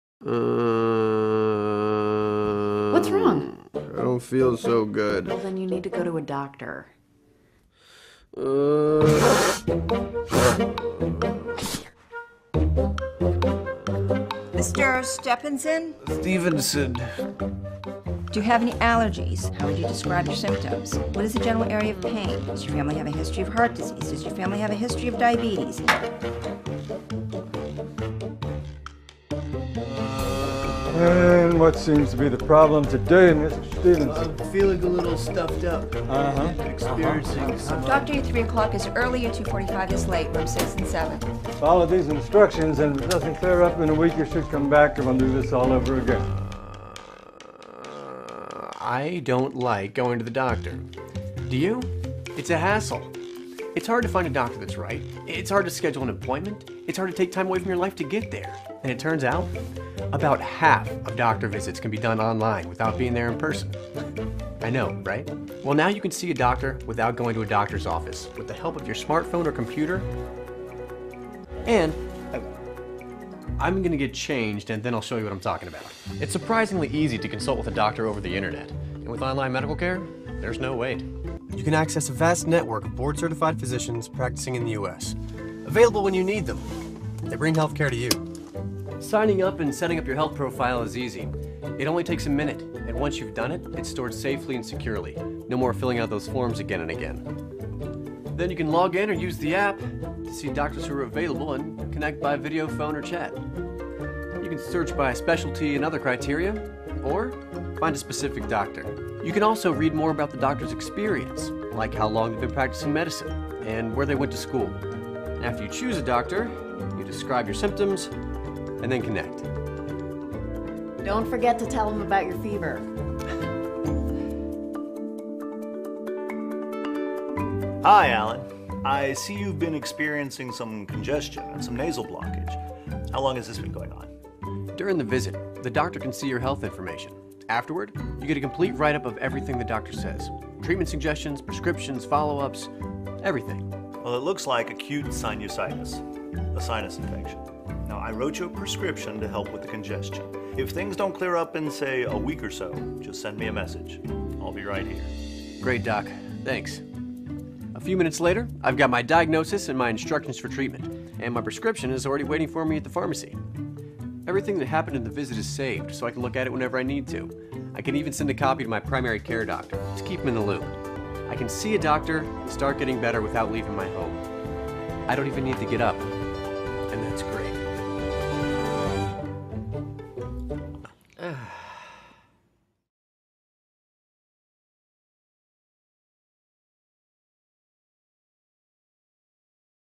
VELNET21 Presentation Ad - R Colley
Why wait days to get an appointment and spend hours in a waiting room when you can talk to a real, licensed MDs.